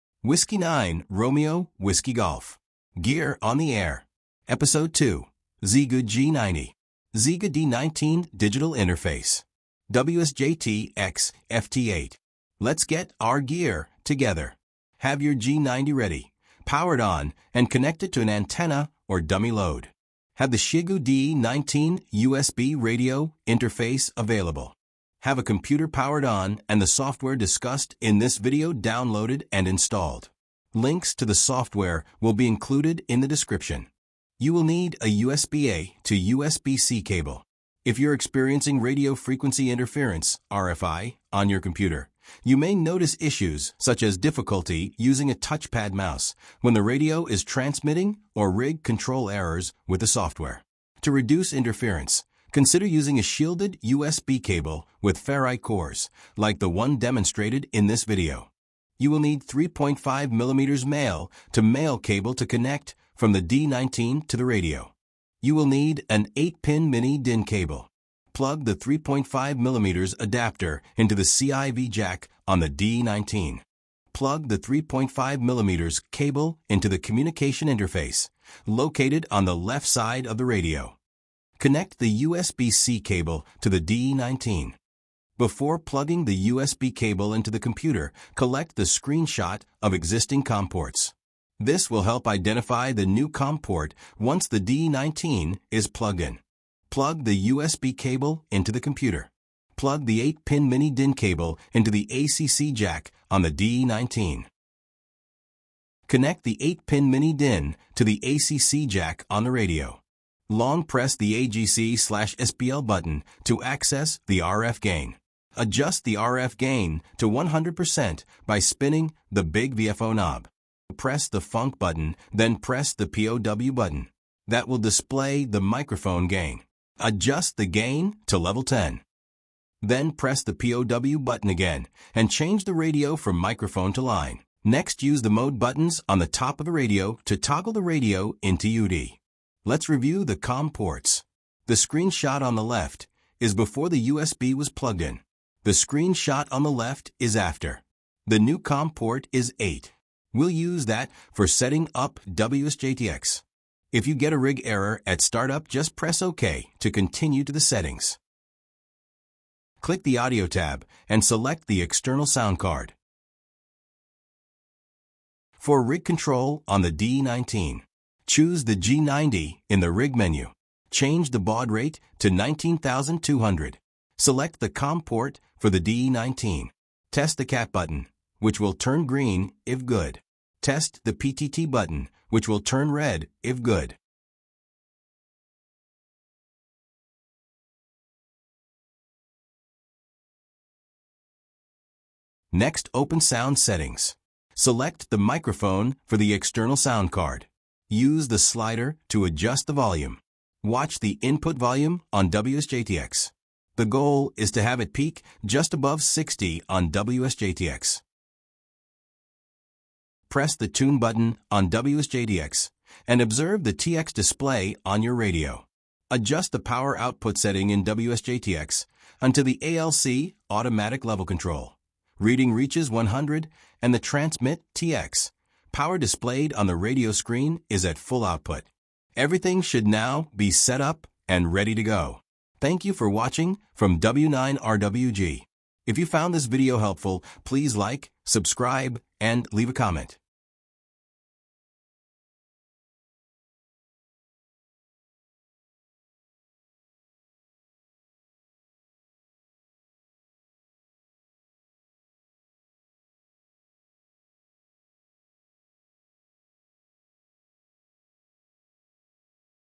G90-DE19 WSJTX
How to connect a Xiegu G90 to WSJTX digital mode with a DE19 interface card
00:00 Intro
00:15 Inventory of items needed
1:08 Connecting Gear to the Radio
1:25 Reminder to Screen Shot Device Manager before connecting USB to Radio
1:37 Connecting Gear to Radio Continued
1:50 Radio Settings
2:20 Computer Settings
3:50  Outro/Making a Contact